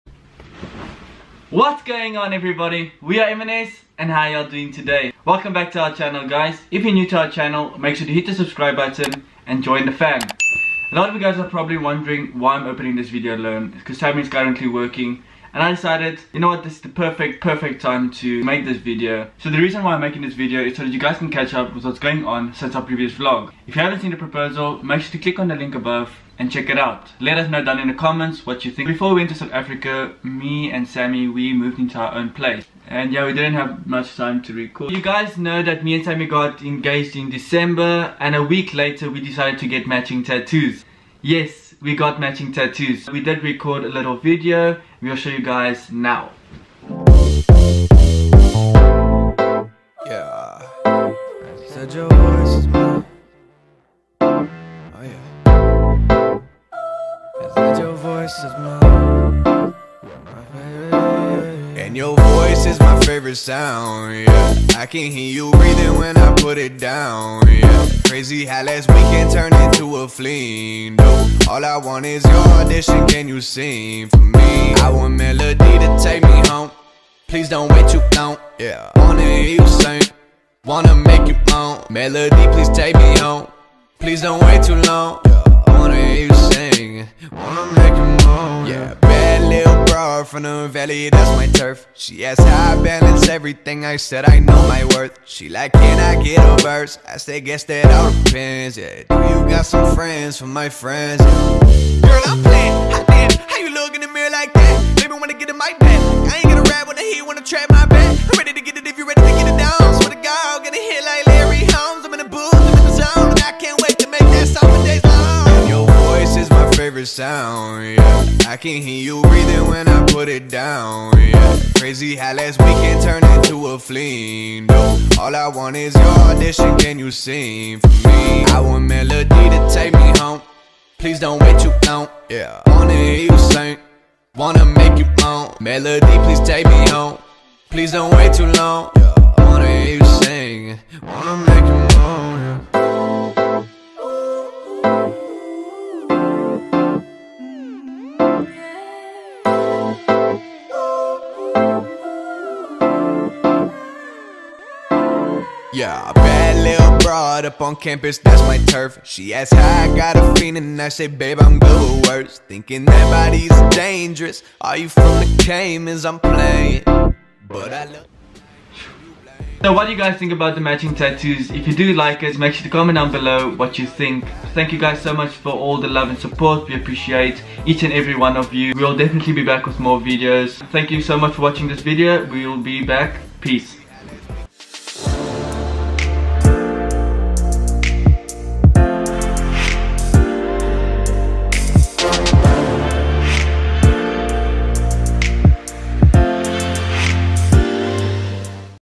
WE GOT MATCHING TATTOOS !😱**SHE PASSES OUT**
Watch us getting our first ever tattoos ltogether (Matching Tattoos)

STALK US :)